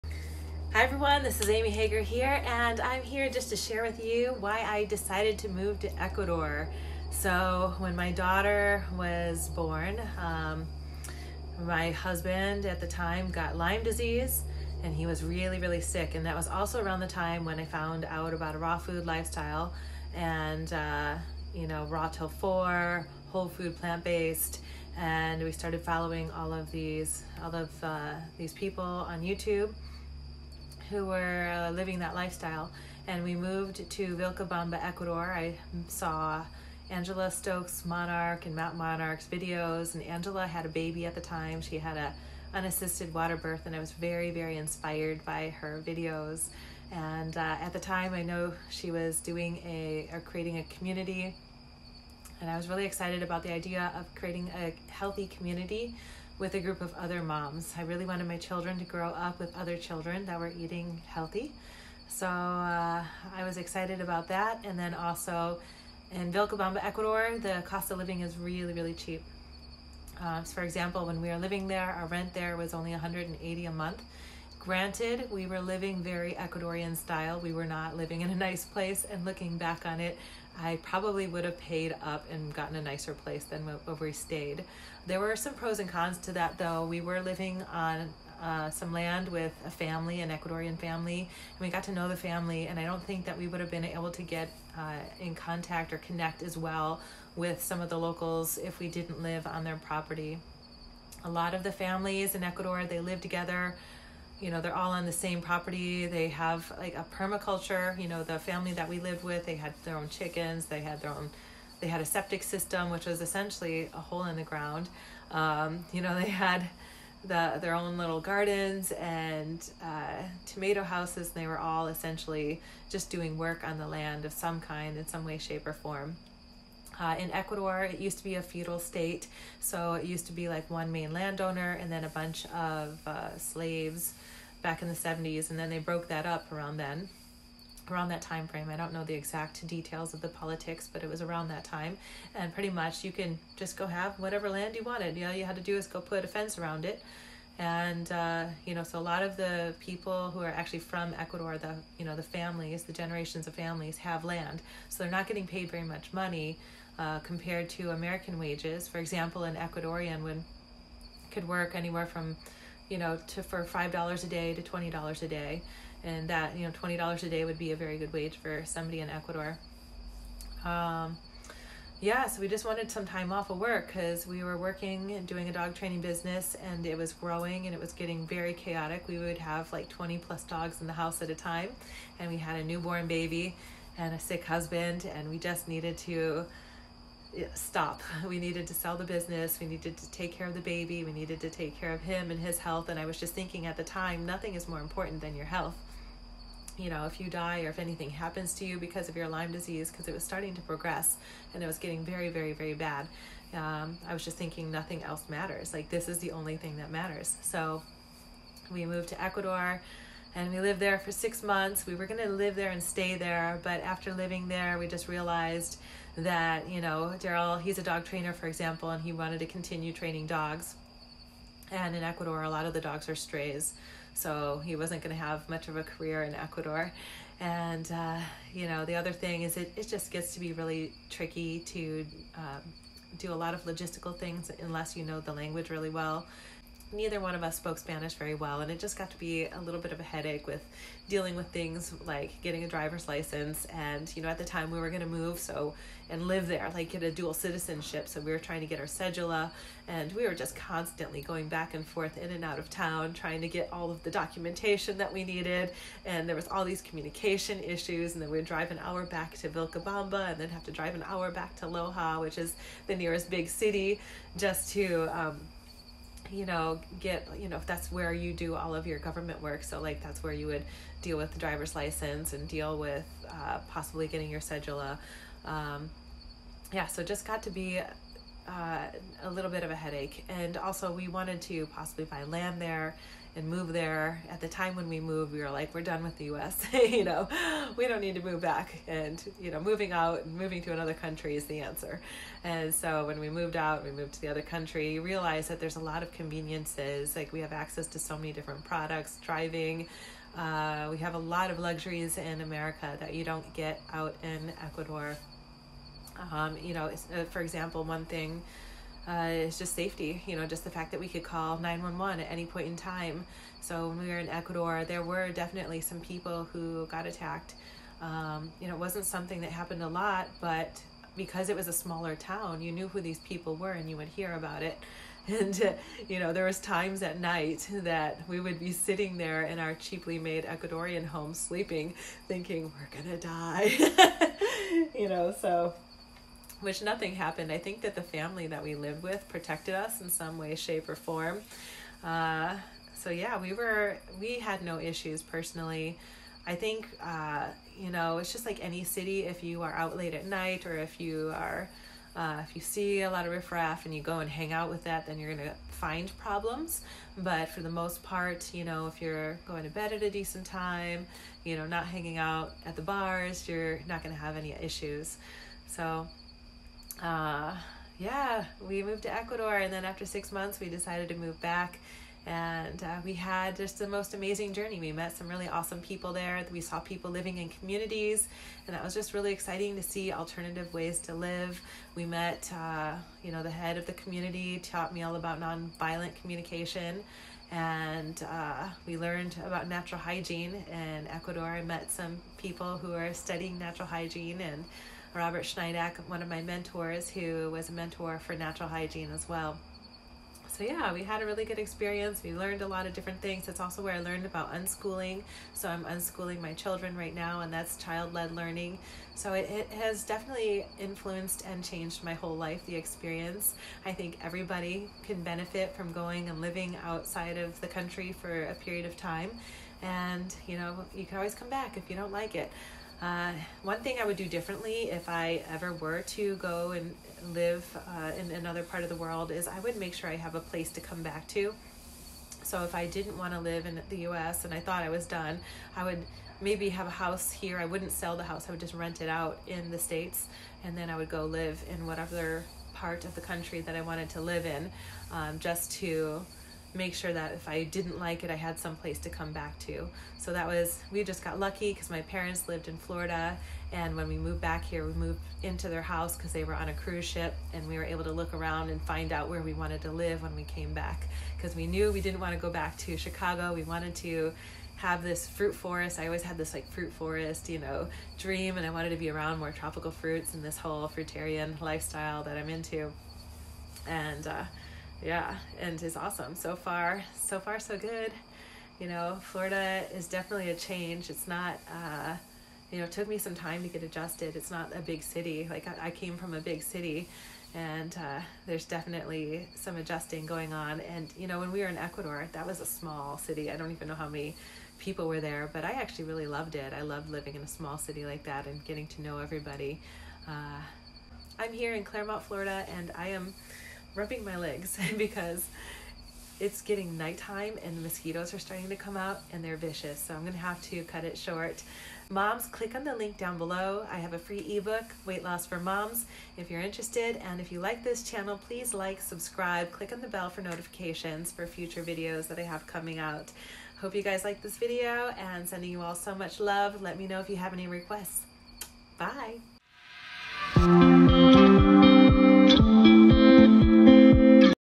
Why I Moved to Vilcabamba Ecuador with My Husband and My 9 Month Old Baby! We Were Done With The US!
Hello Everyone,

This video is about why my husband, our nine month old baby and I moved to Vilcabamba Ecuador!

Much Love!

Amy